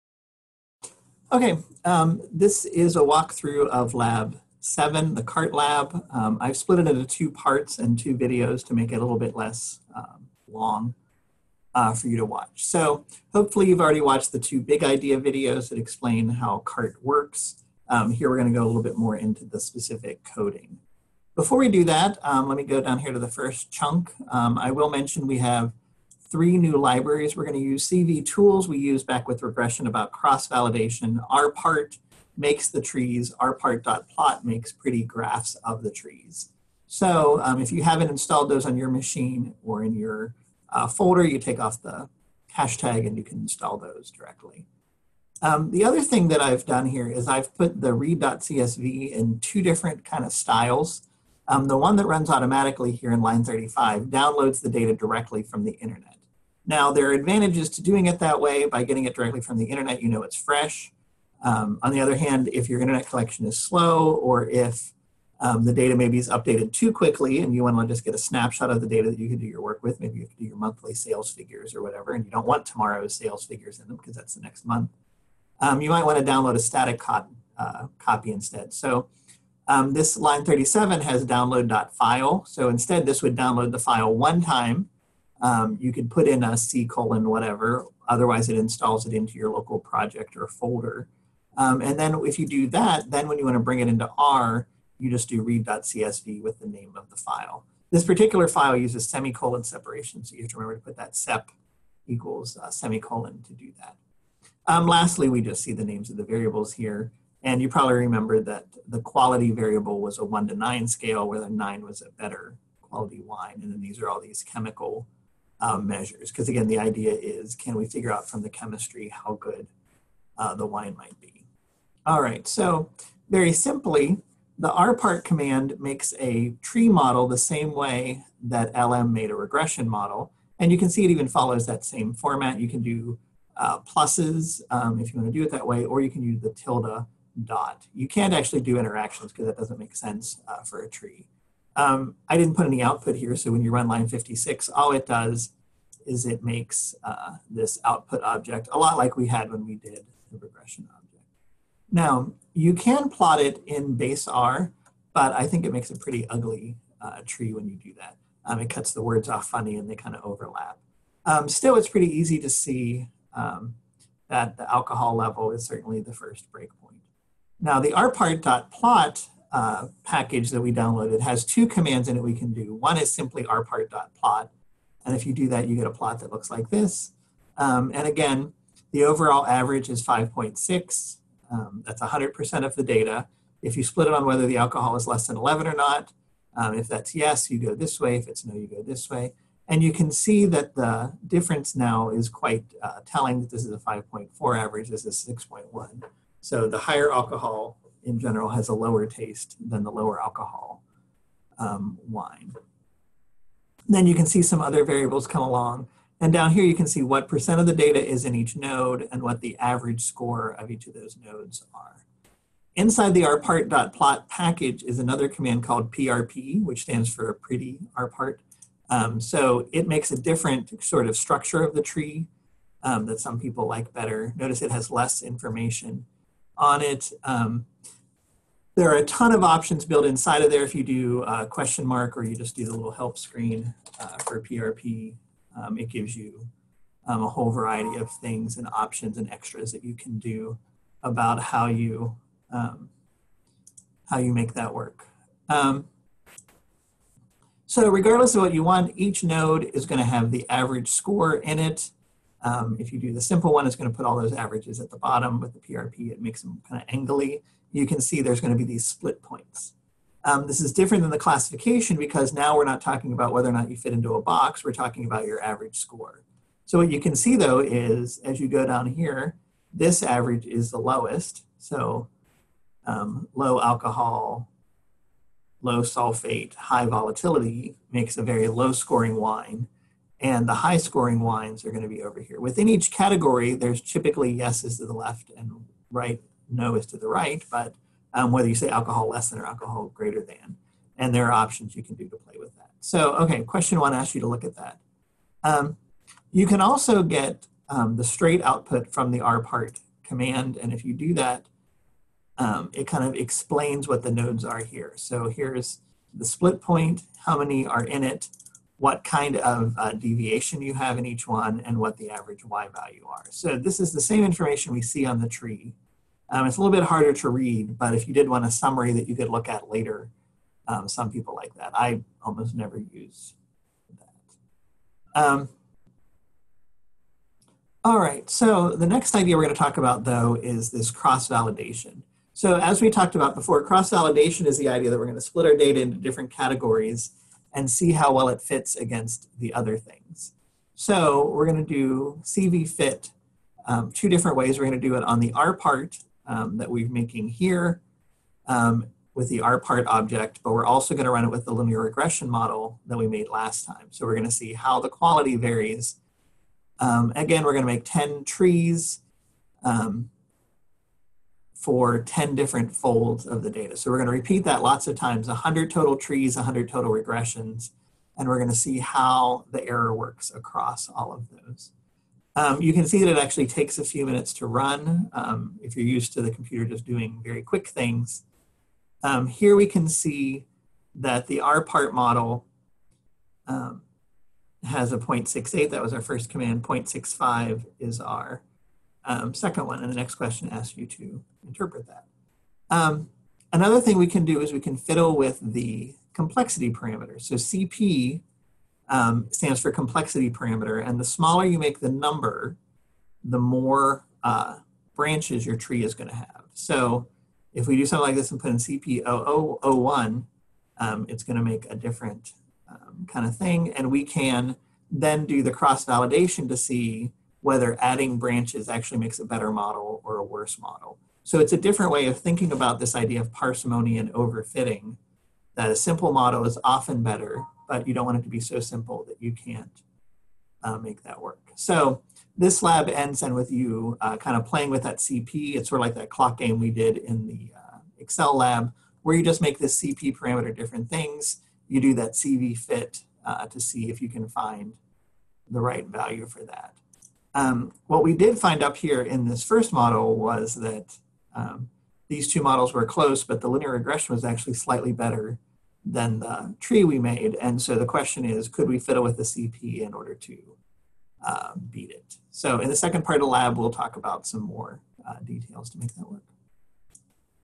Lab 07a: CART and rpart
This is part one of a walkthrough of the CART lab.